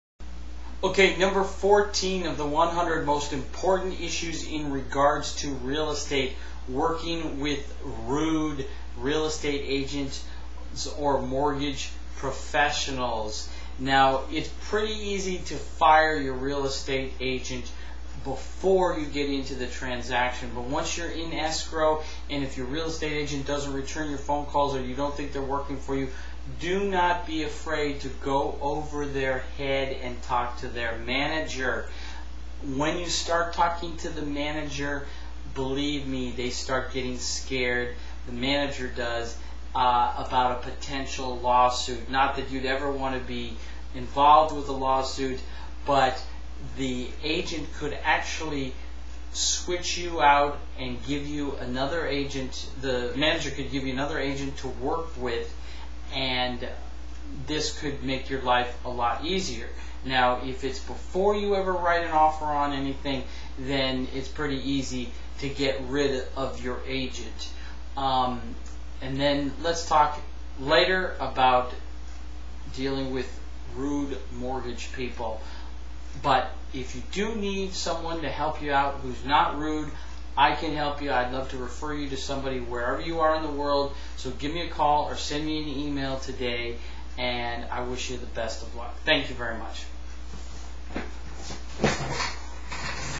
100Most Important RealEstateIssues14 RUDE AGENTS
Jim Meyer from RE/MAX Gold in Fairfield California talks about the 100 most important real estate issues when buying or selling a home in America today!
1-888-JIM-7888